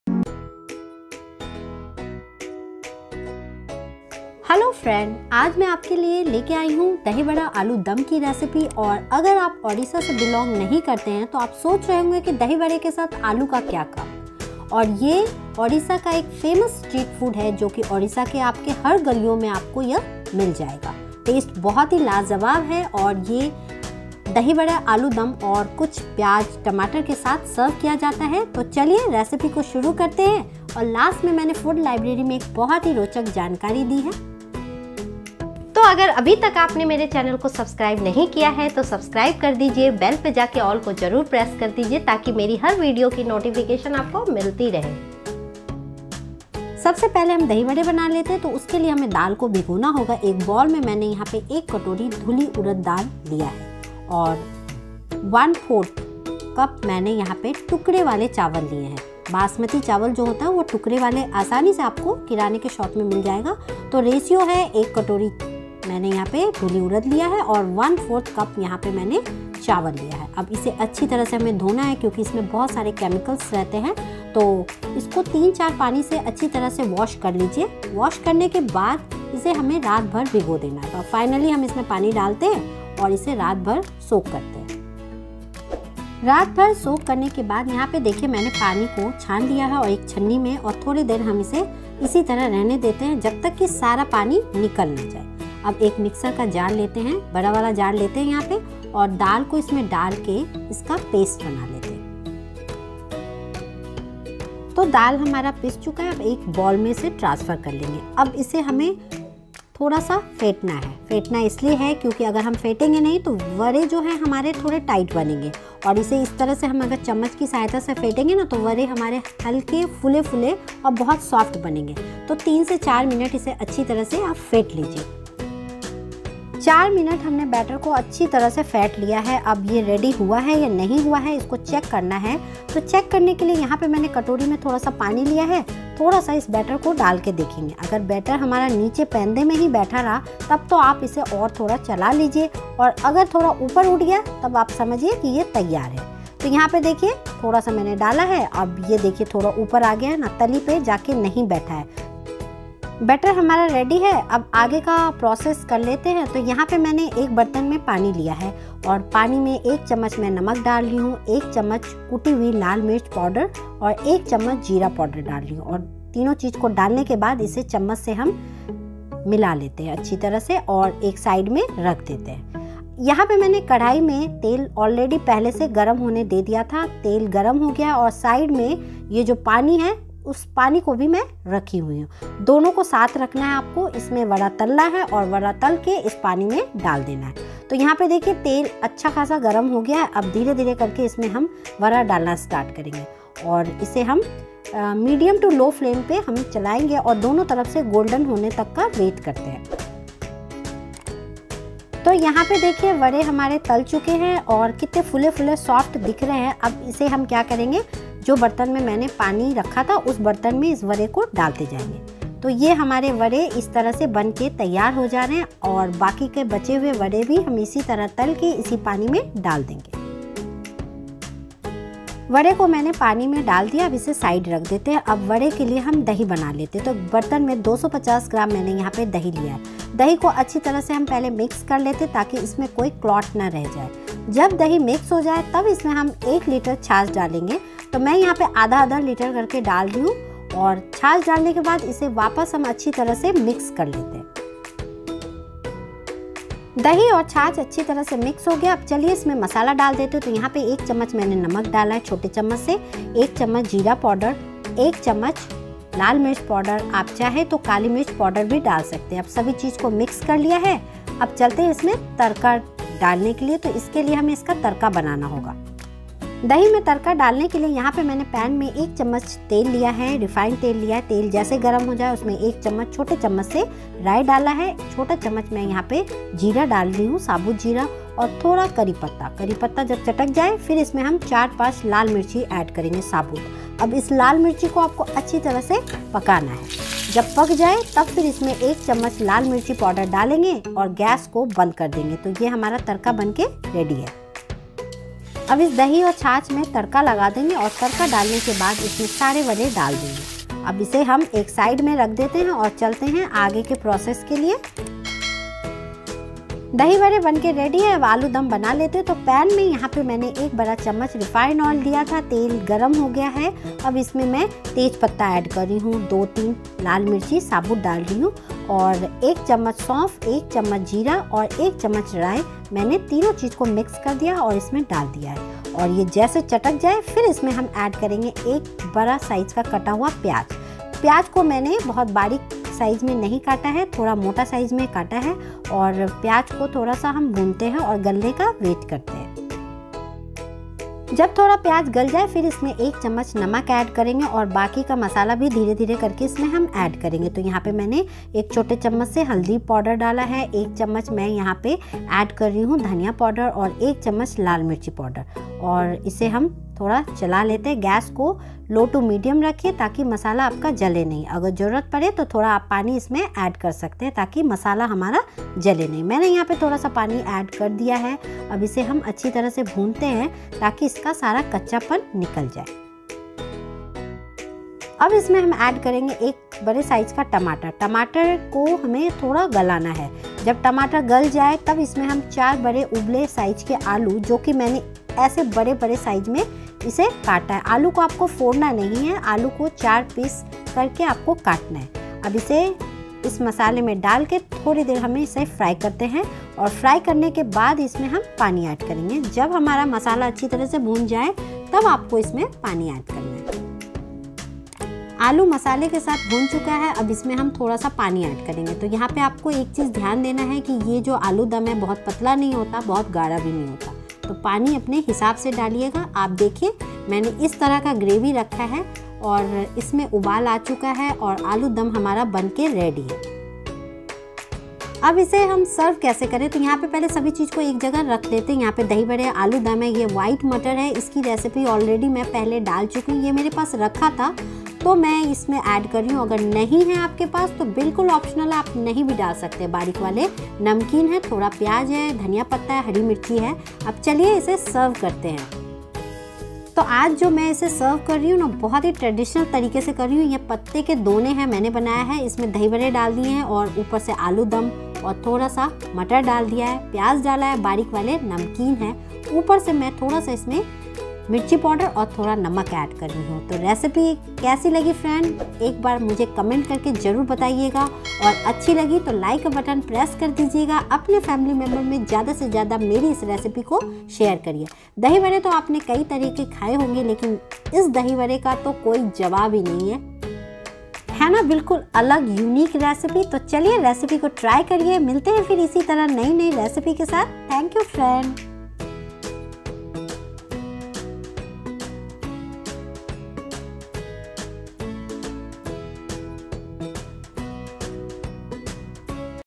Dahi Vada Aloo Dum Recipe l Odisha Special DahiBara Aludum l ओड़िशा की चटपटी दही वड़ा आलू दम चाट
दही बारा, आलू दम के बारे में - ओडिशा का प्रसिद्ध स्ट्रीट फूड पकाने की विधि

ओडिशा का एक प्रसिद्ध और अनोखा स्ट्रीट फूड। नरम दही वड़े के ऊपर मसालेदार आलू दम और भुना जीरा और मिर्च पाउडर छिड़का हुआ है। आपको सड़कों पर साइकिल के दोनों किनारों पर लटकी हुई बड़ी हांडी (बर्तन) वाले विक्रेता मिल जाएंगे।
आवश्यक सामग्री Ingredients:

For vada

Dhuli urad daal 1 cup
Broken rice 1/4 cup
Oil for fry
For tarka
Oil 1 tbsp
Jeera , mustard seed 1 tsp
Red chilli 3-4
Curry leaves
Chilli powder 1 tsp
Curd 250 grm
Chaas 1 litre

For aloo dum

Boiled aaloo 4
Tomato 3
Onion 1
Oil 1 tbsp
Jeera ,mustard seed , saufh 1 tsp
Haldi , mirch powder , corrinder powder 1 tsp
Salt as per taste
Water